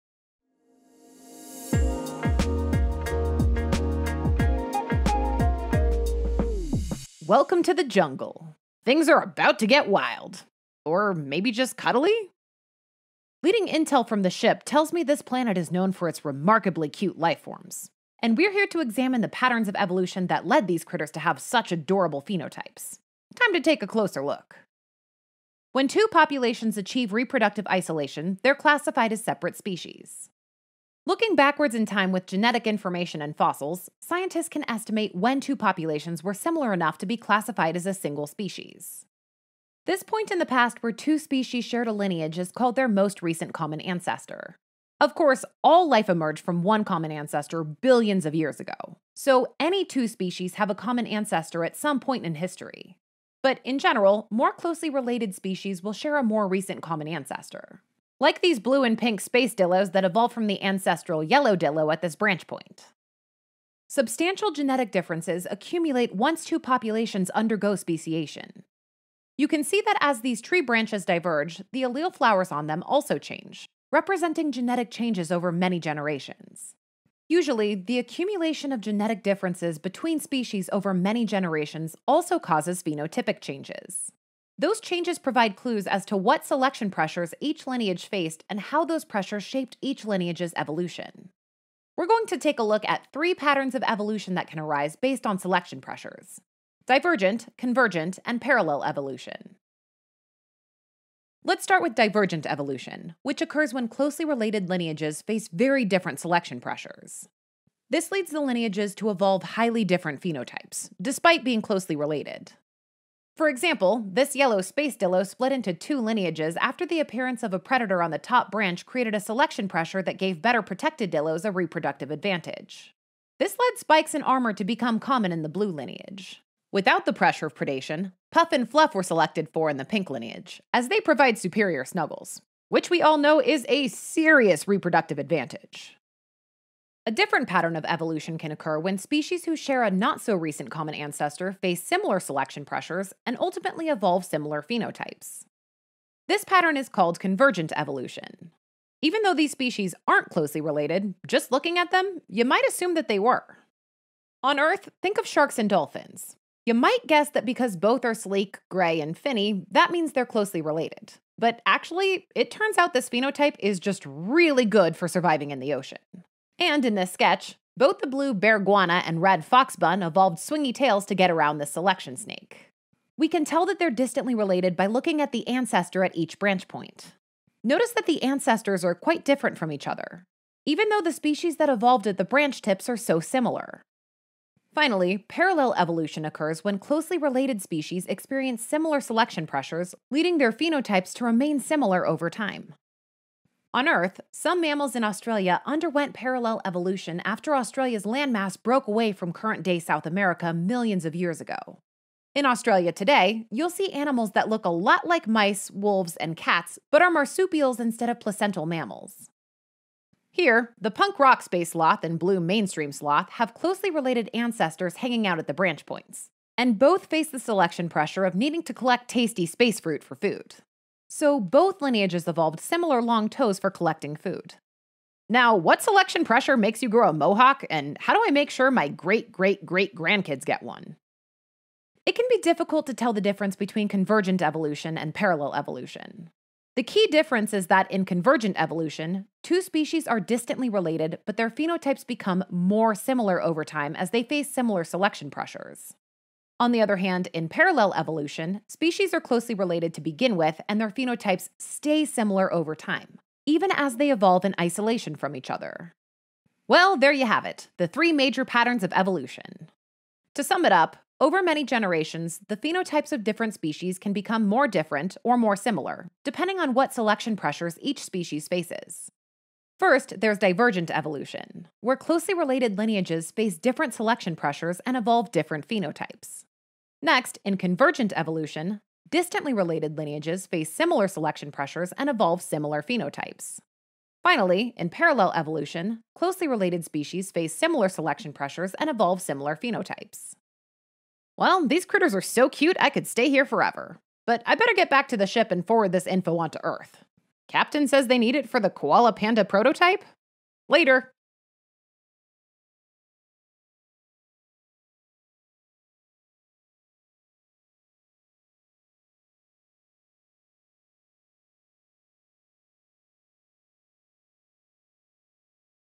Patterns of Evolution: Divergent, Convergent & Parallel (Full Lesson) | Sketchy MCAT
Learn the patterns of evolution: divergent, convergent & parallel. Learn about reproductive isolation, most recent common ancestor, and deciphering species traits.

00:00 Introduction
00:25 Species
00:40 Most Recent Common Ancestor
01:03 Speciation
01:31 Divergent Evolution
02:16 Convergent Evolution
03:04 Parallel Evolution
03:51 Difference between Convergent and Parallel Evolution
04:16 Symbol Review

Join over half a million students who became doctors with Sketchy!